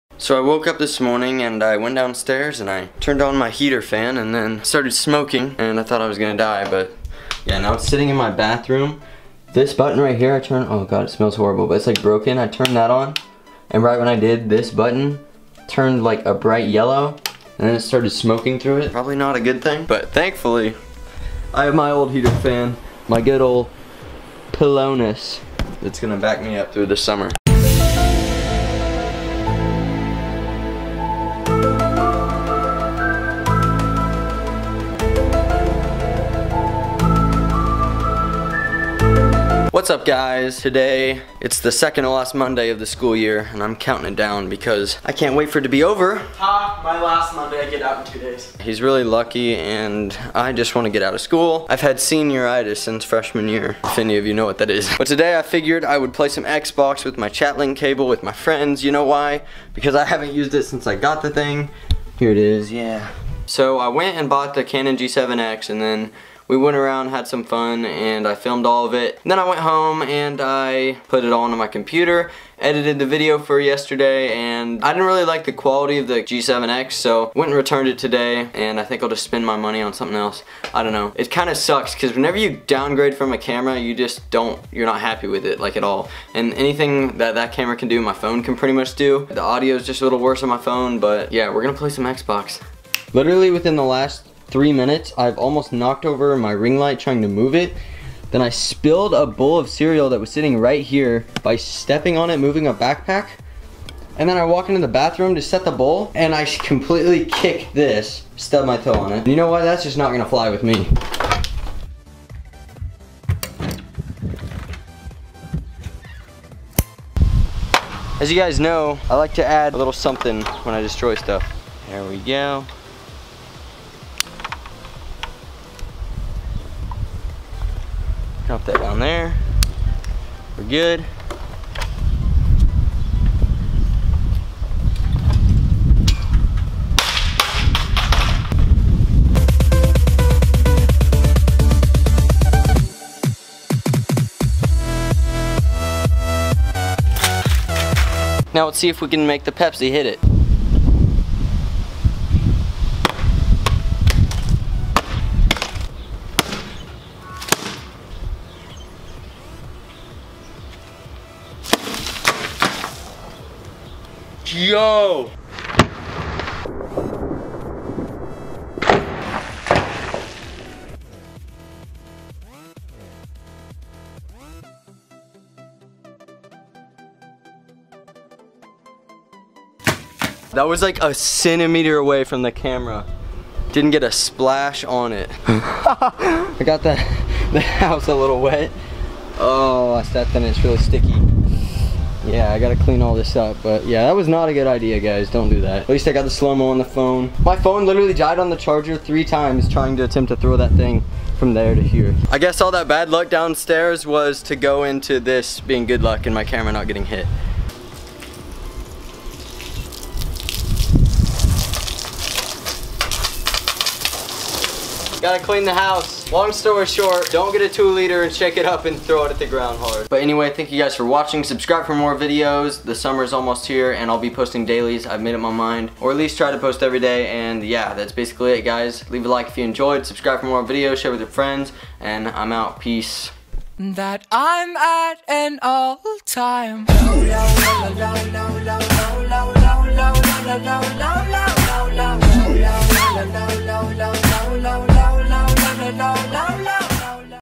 THIS WAS A BAD IDEA...
About broke the window or the camera or both... Oh well lmao

Snapchat: Tanner_B24 (Random moments from my daily life)
Twitter: @Braungardtanner (I follow back :)

If you've read all of this you're awesome ;)